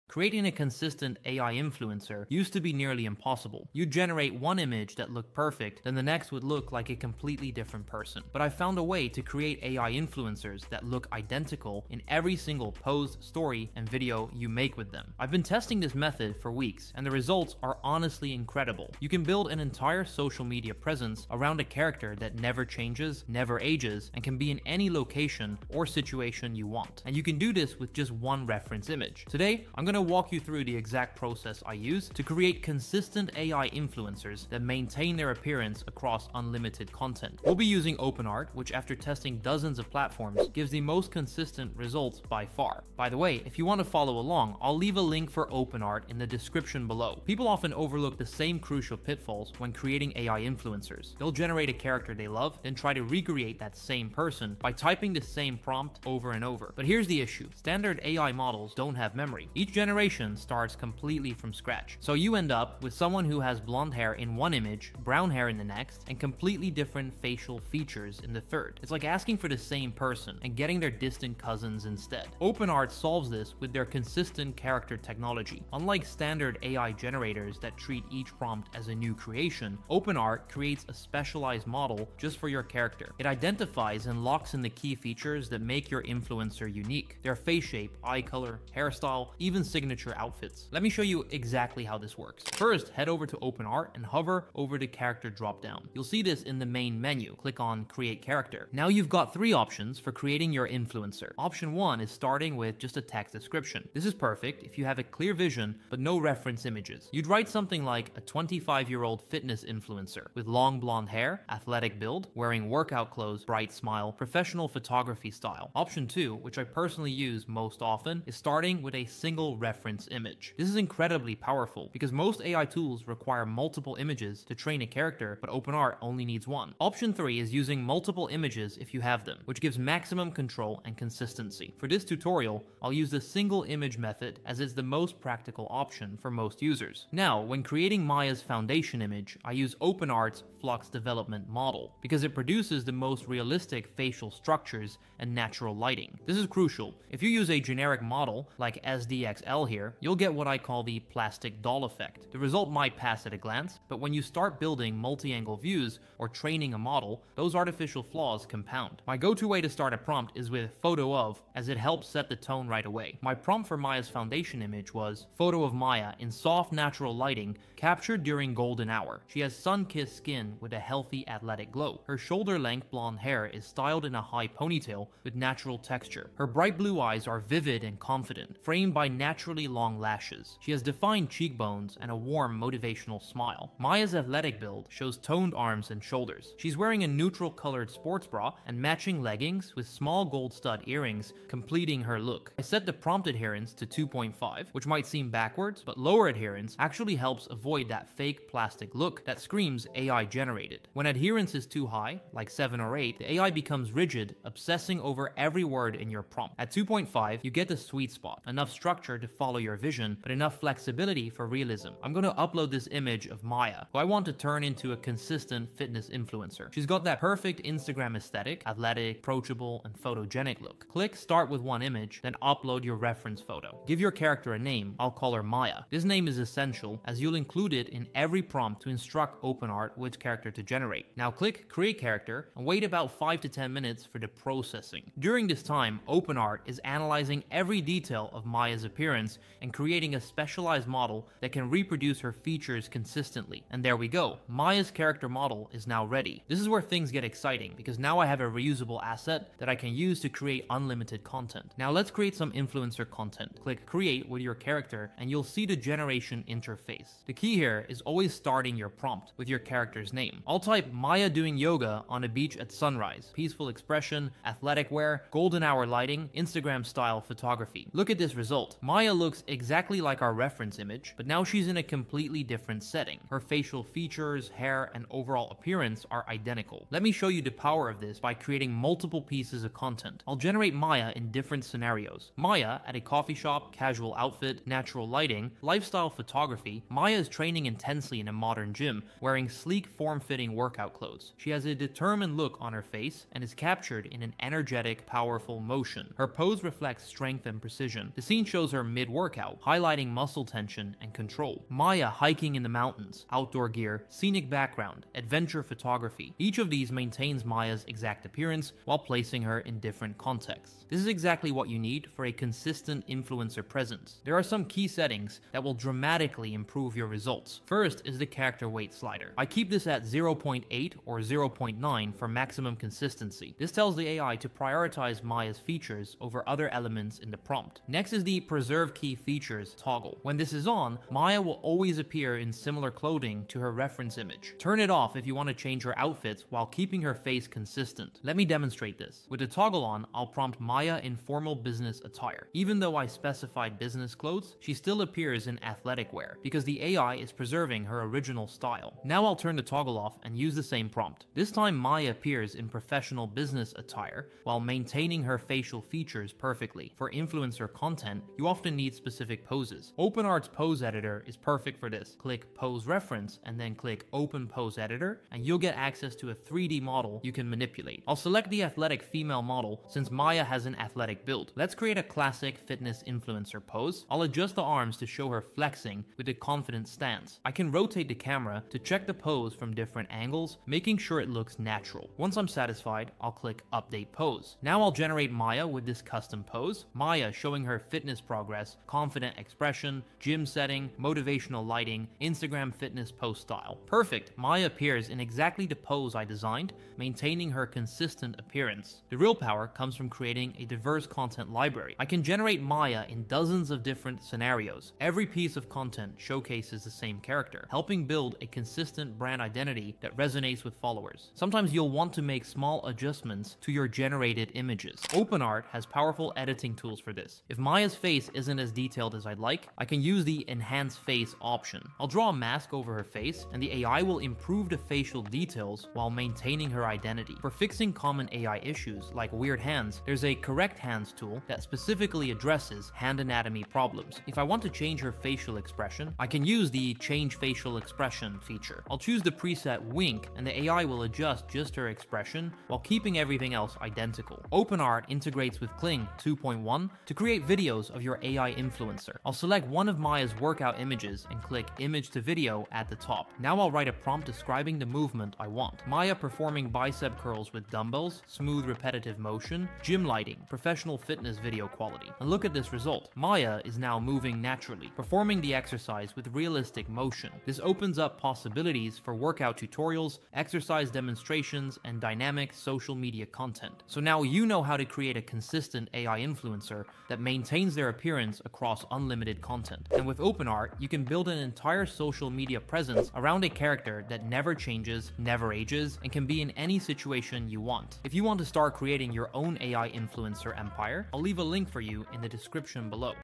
How to Make Consistent AI Influencers You Can Monetize (Step by Step)
in this ai influencer video I will show you how you can make your own ai influencers which you can then monetize through various different monetization strategies!